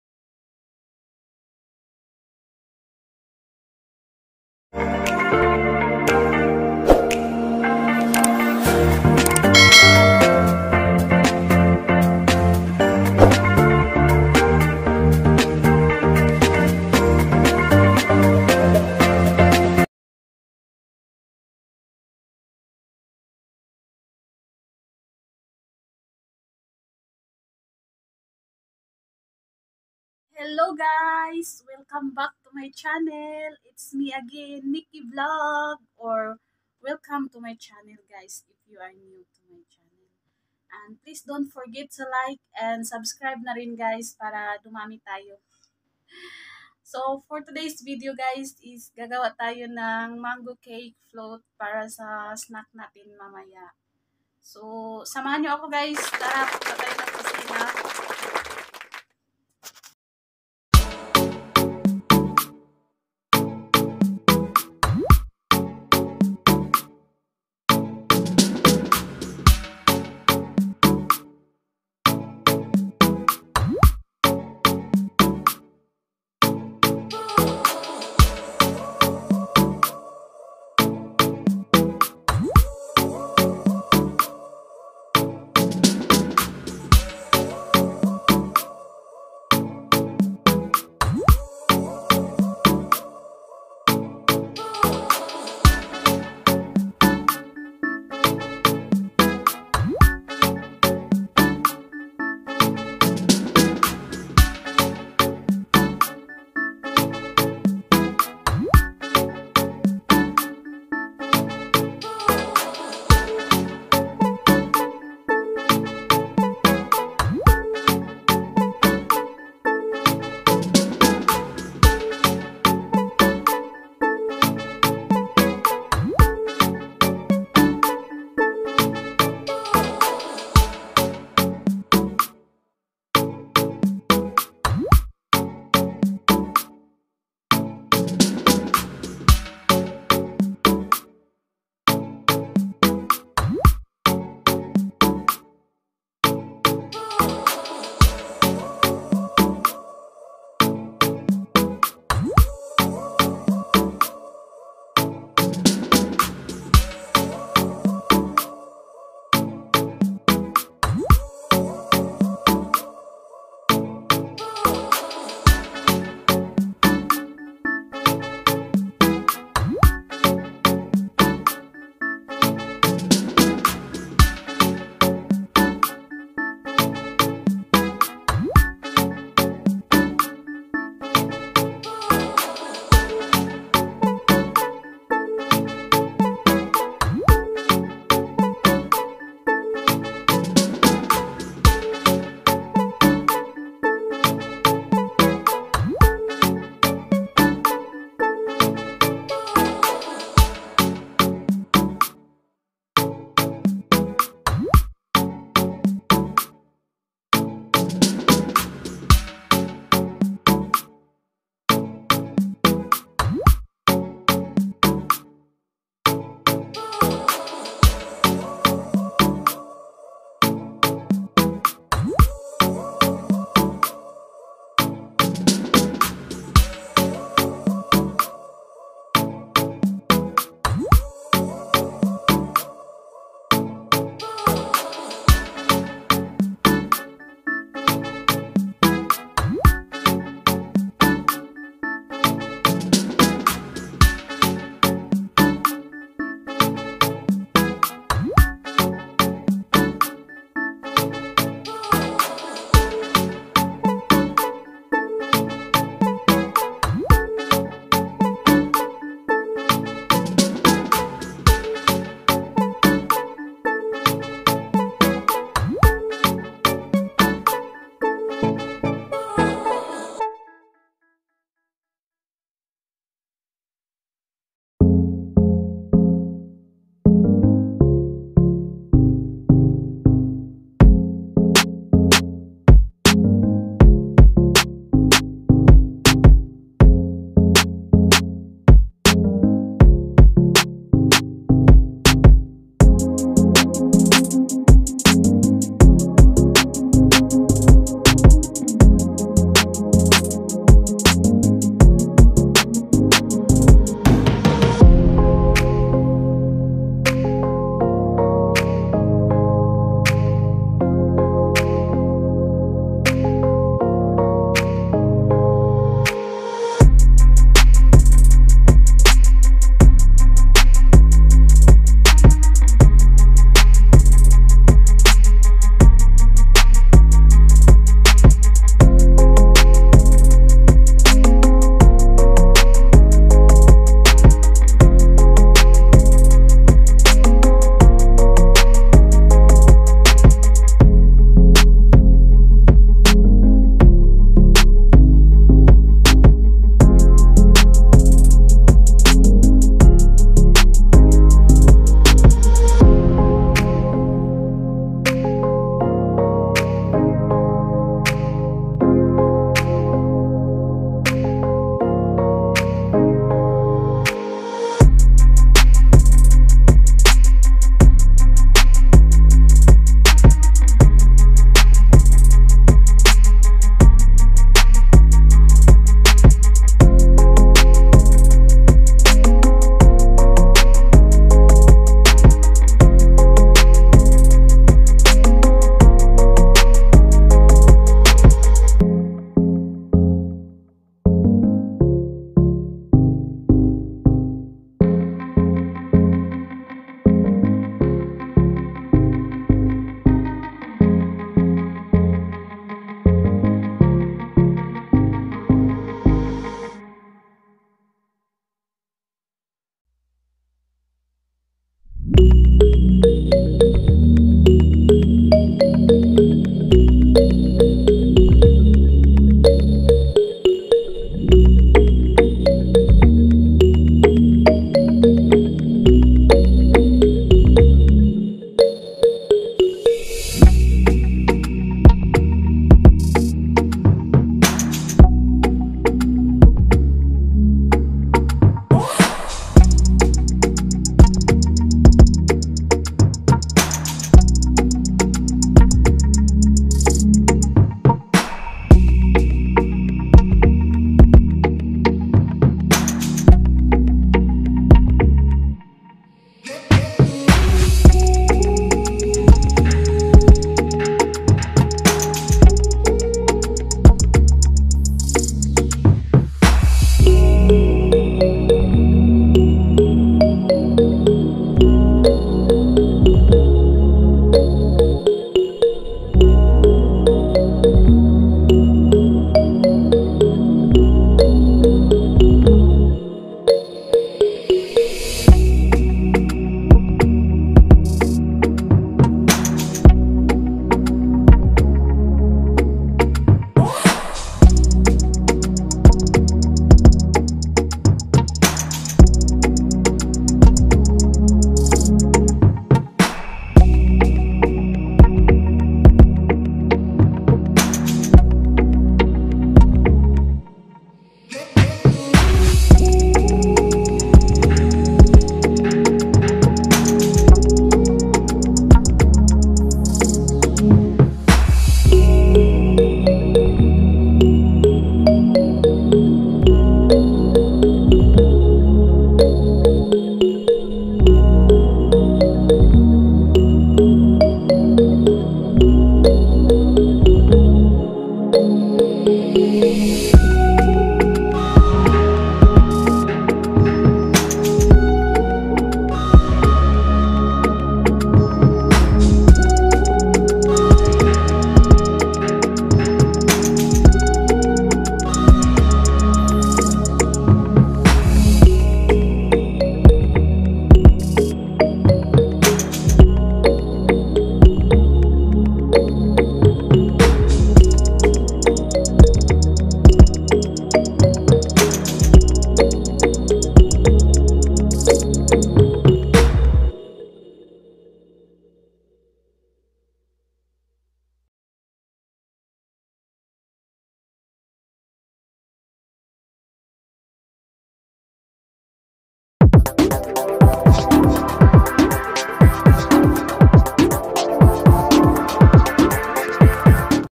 How to make Mango cake float/simple recipe/Perfect for snack/MiQui Vlog
Ingredients:
1kg mango pulp
1tbs cornstarch
1glass water then boil it in 5minutes
1pck pound cake
for toppings:
1can condensed milk
1can nestle cream
1pouch kdd liquid cream or any liquid cream
2pouch thick cream
1pouch dream whip
5pcs Kiri cheese